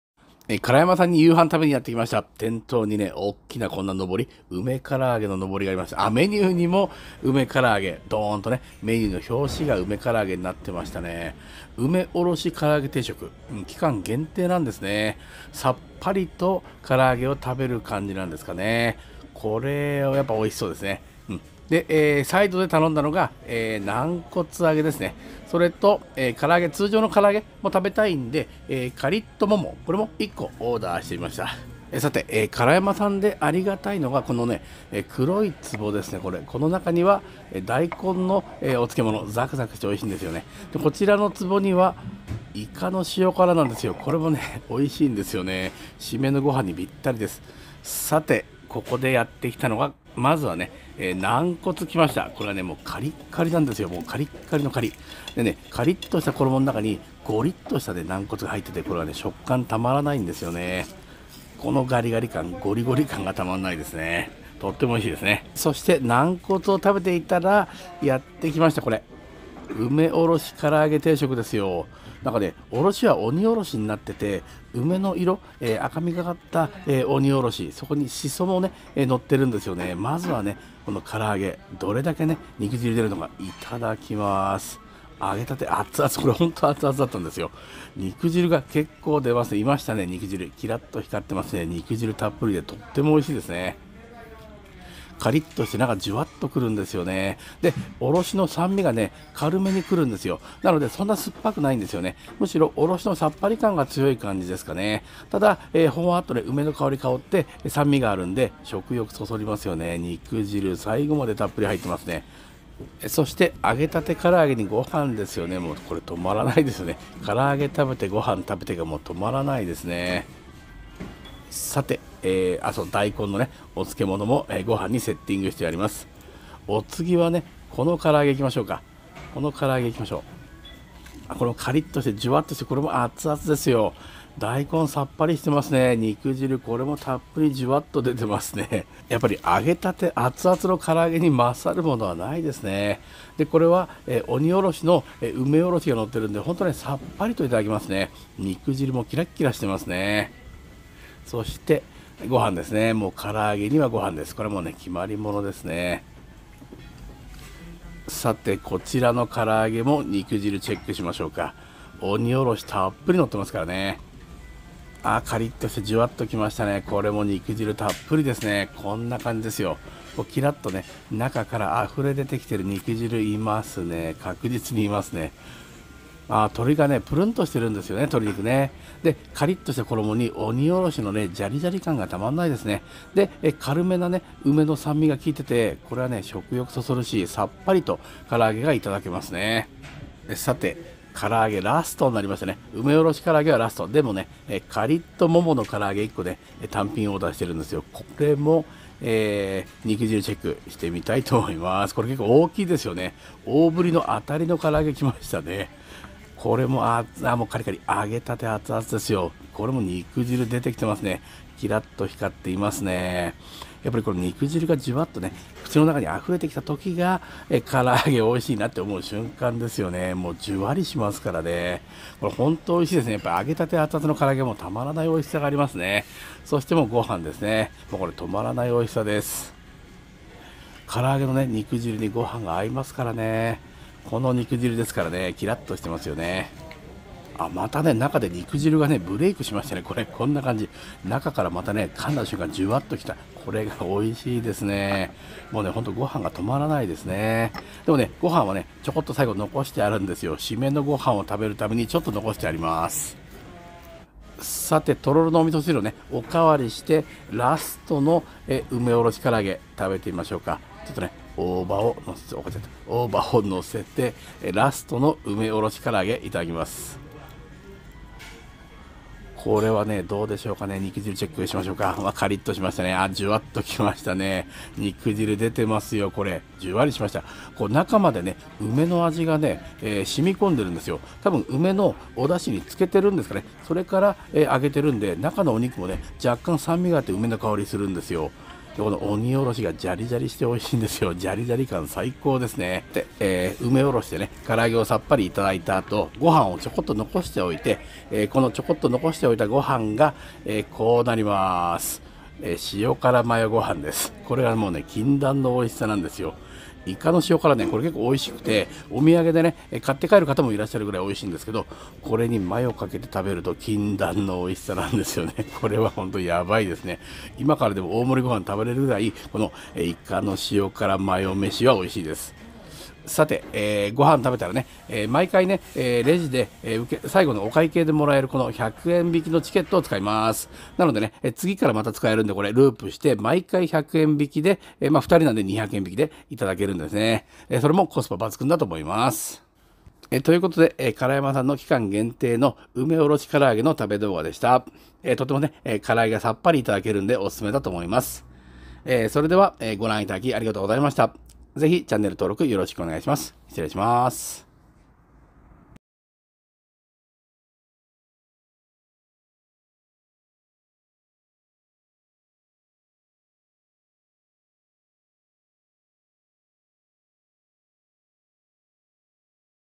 さっぱり美味しい！梅おろし唐揚
ご覧頂きありがとうございますm(__)m

からやまさんで期間限定の梅おろしからあげを食べてきました。

ほんのり梅の爽やかな酸味が染みたからあげには梅の鬼おろしがたっぷり！
肉汁たっぷりのからあげをさっぱりと美味しく食べることができました。